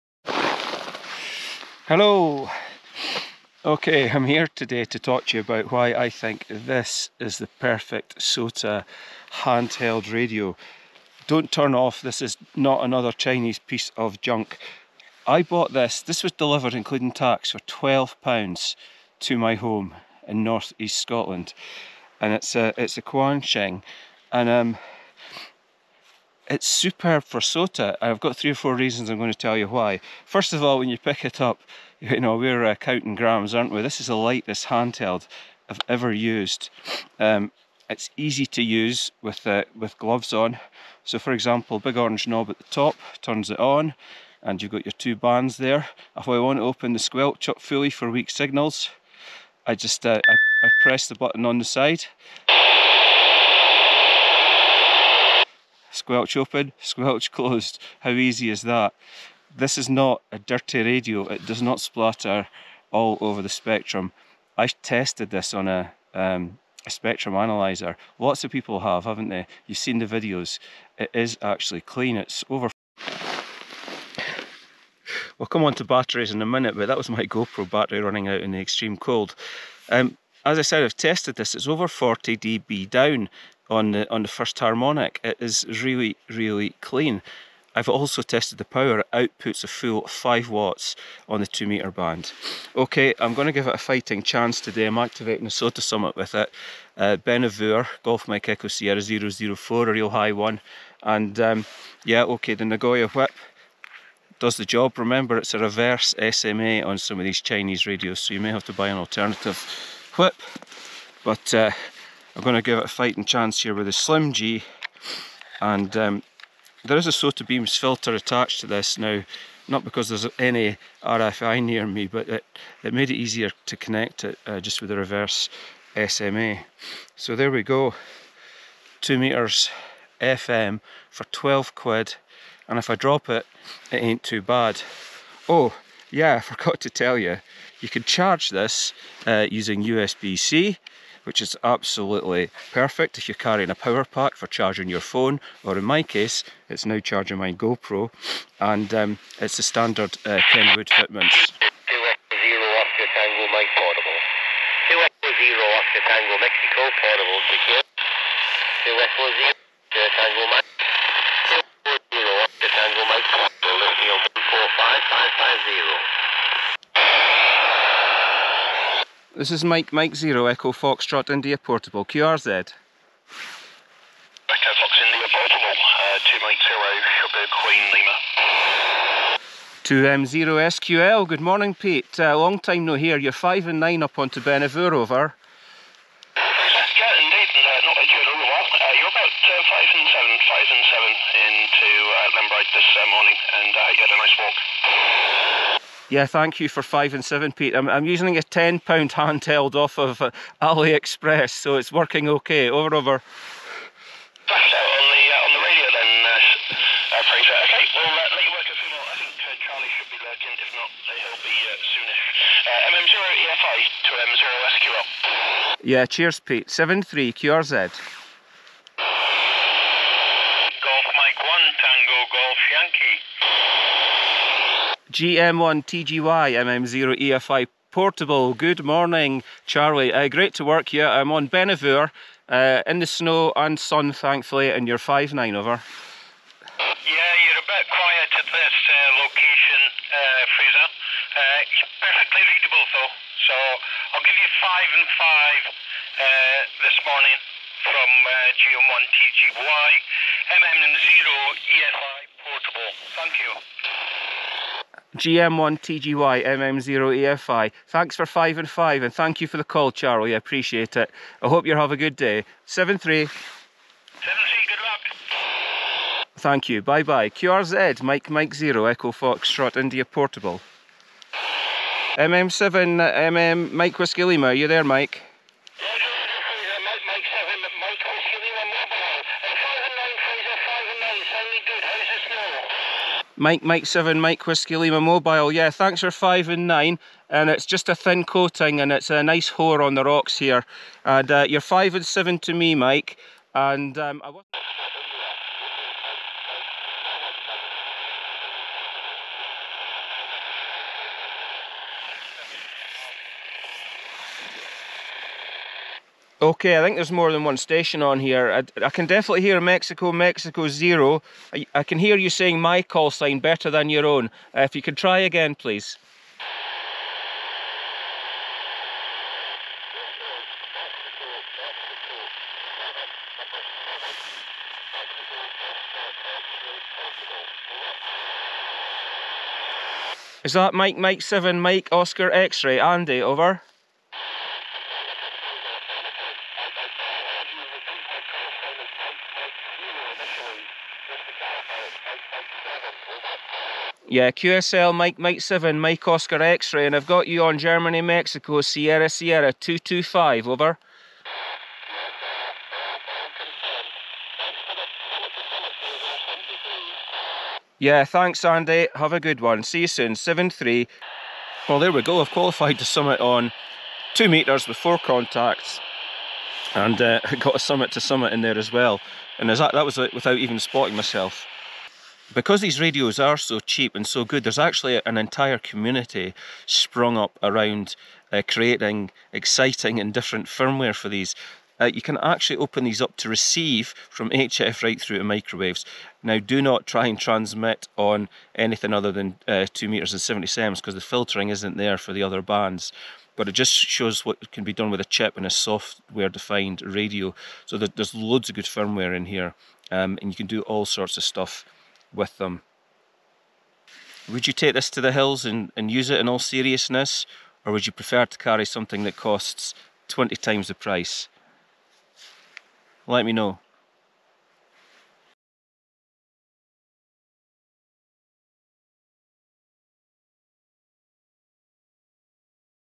Is the best Hand Held HT for SOTA? - Testing the Quansheng on the bench and a summit
The Quansheng UV-5K

This radio cost me £6, but £12 with taxes and delivery. It took six days to arrive.

I tested it on the bench:
Into a VSWR meter and dummy load - output is 4.8w, 2.5w and 2.5w on high, medium and low power respectively.

Using low power and 60dBm of attenuation, I measured it -42dBm down on the first harmonic. There are further harmonics, and the largest of these is at 420MHz. It is also 40dBm down on the fundamental frequency.

The Quansheng UV-5K has a wealth of support via different firmware providers. Don't be afraid to try one. If you brick the radio, you will be able to re-install the factory firmware to get the radio going again.